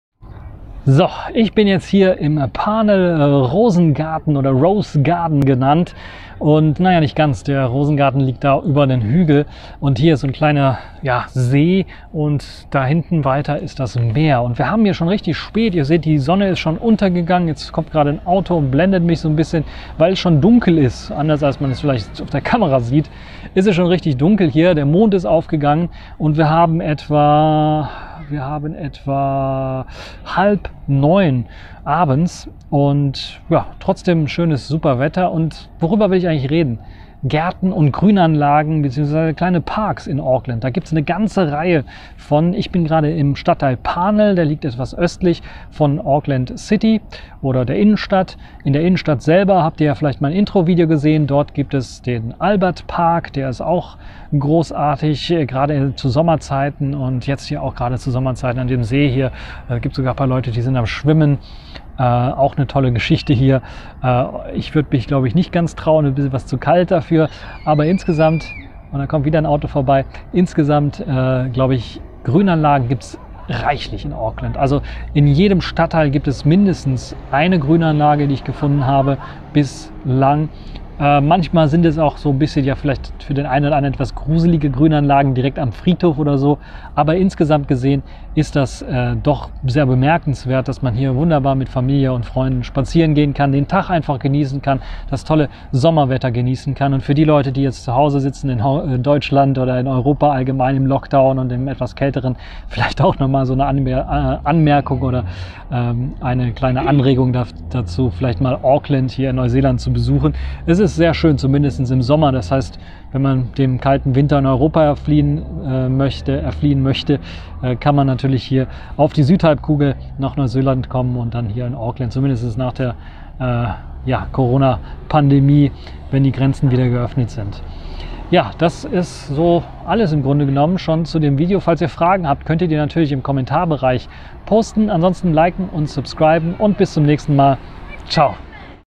Parnell Rosengarten in Auckland
Ich stelle euch den Parnell Rosengarten etwas näher vor, der gerade zu Sommer zu einem Spaziergang anregt um die Natur zu erleben.